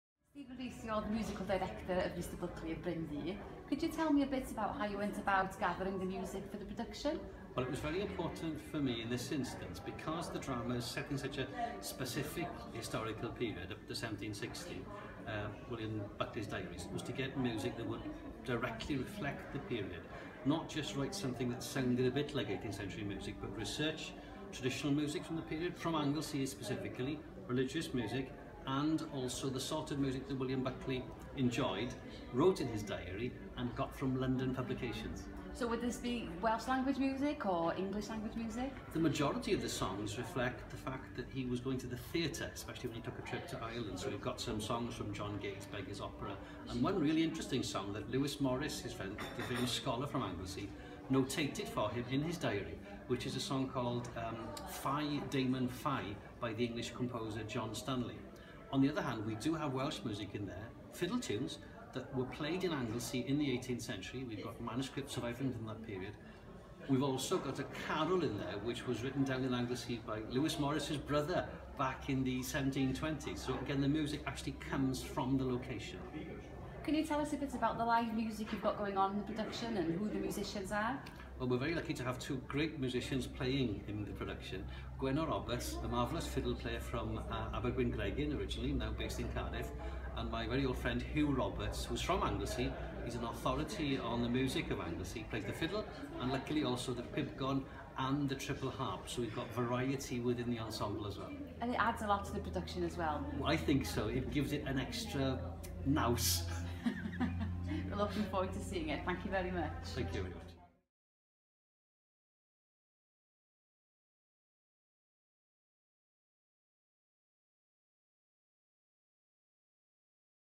Interview with Stephen Rees - Mr Bulkeley o'r Brynddu Drama
Interview with Stephen Rees, the musical director of Mr Bulkeley o'r Brynddu drama.
Mr Bulkeley o'r Brynddu
Theatr Bryn Terfel, Pontio
Tuesday 26 April 7.30pm